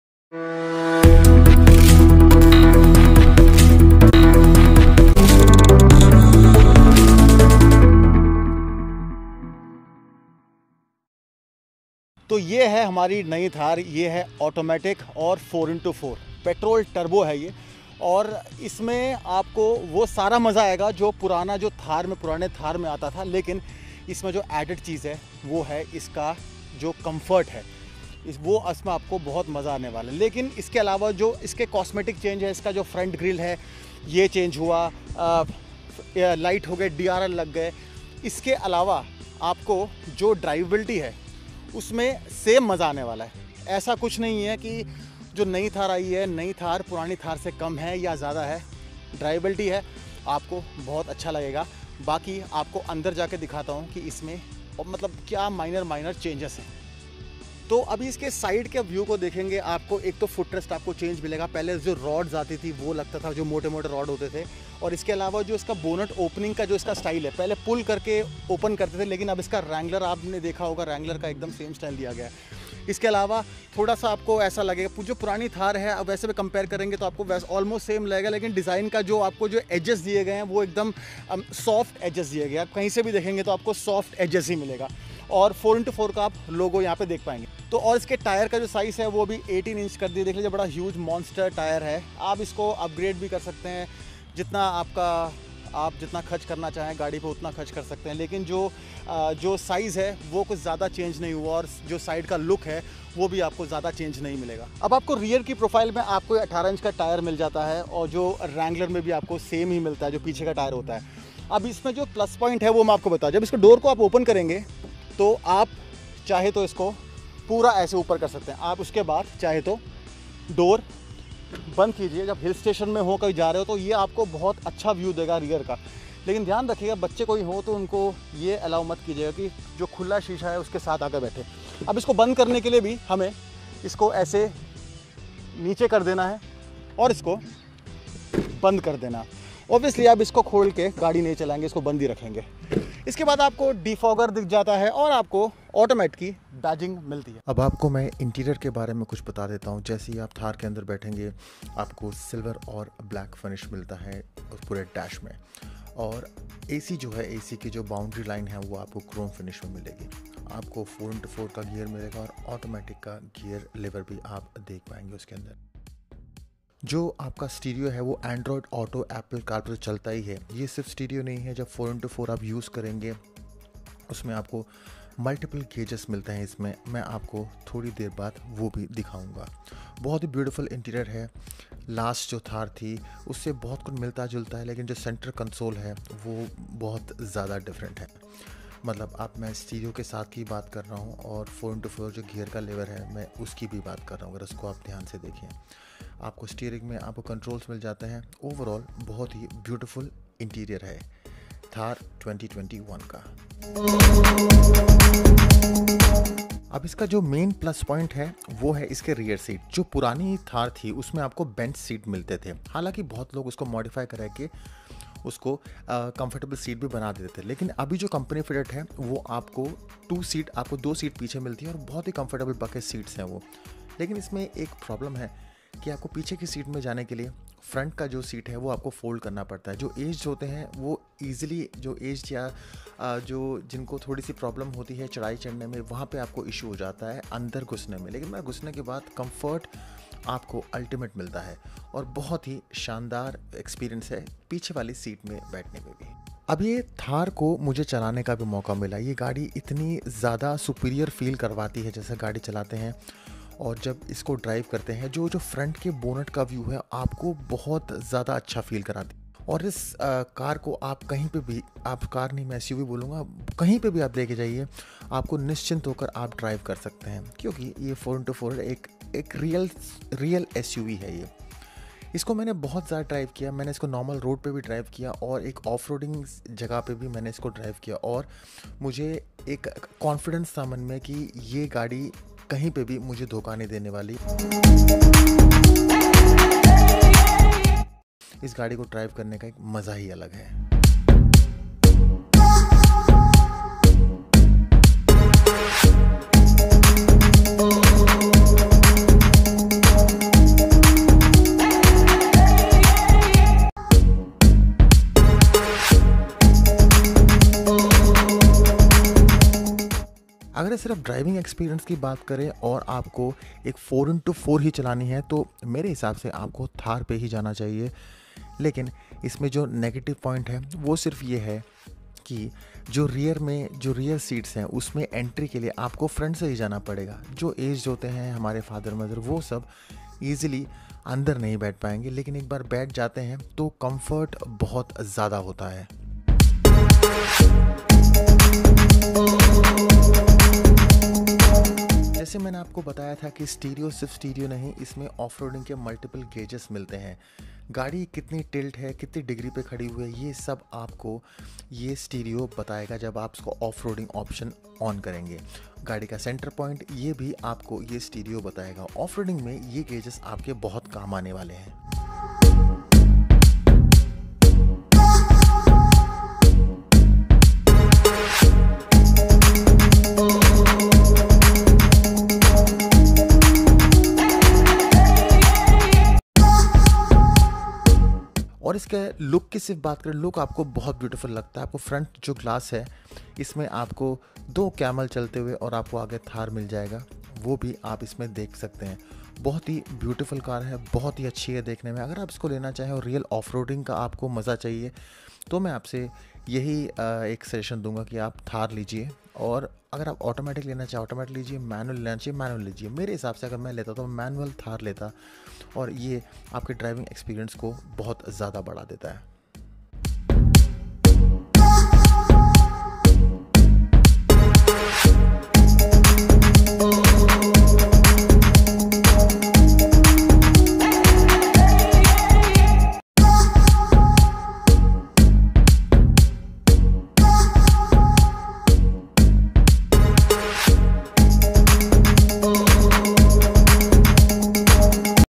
New Thar 2021 Automatic. Drive review
A small video on Mahindra Thar Petrol automatic . drive review

Thar 2021 Interior and exterior details